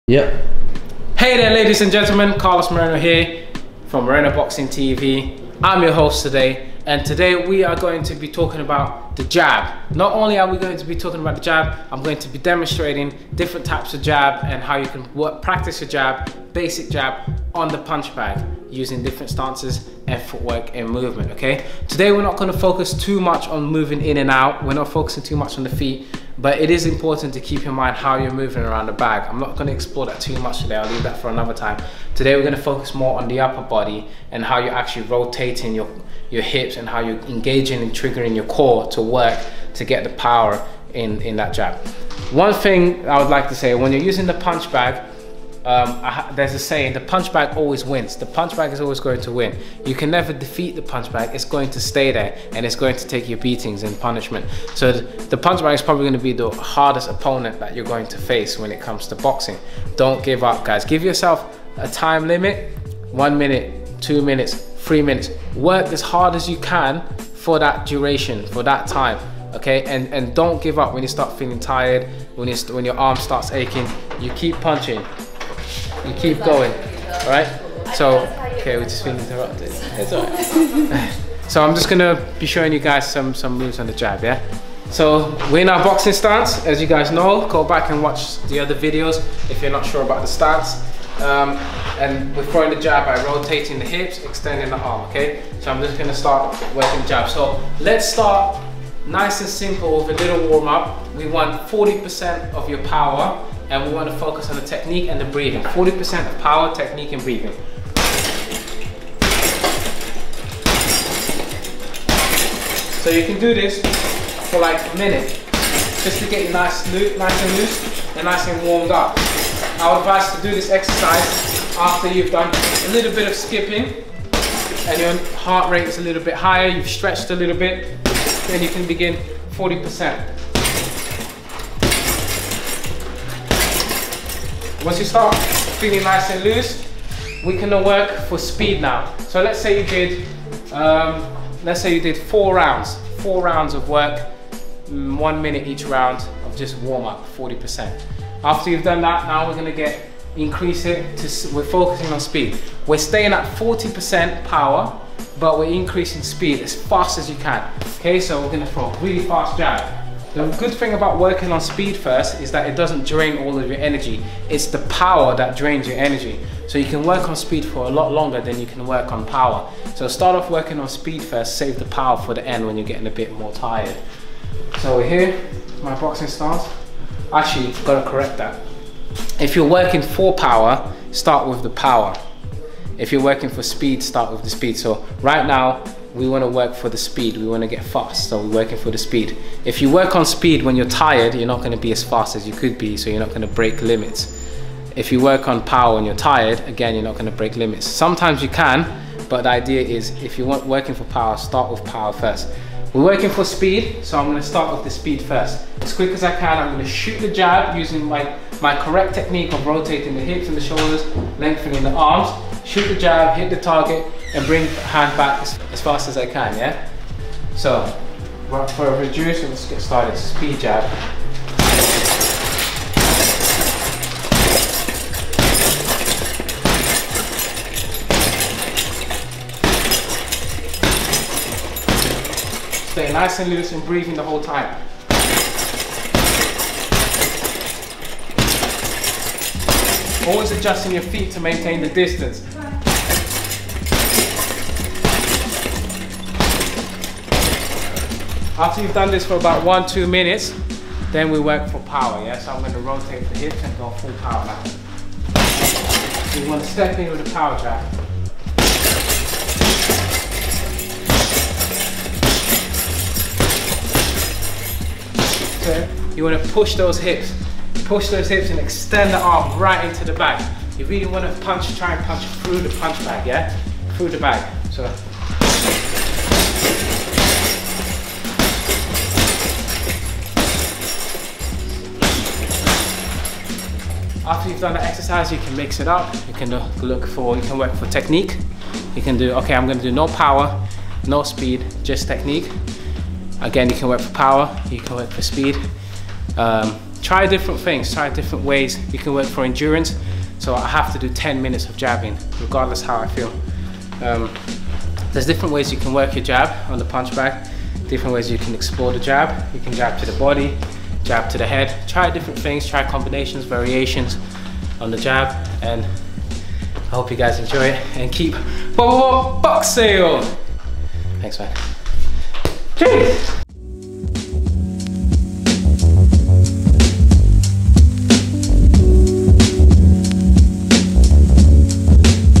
How To Throw Different Jabs in Boxing
In this video Carlos Moreno teaches you how you can throw and practice different types of jabs on the heavy bag.

Instagaram @morenoboxing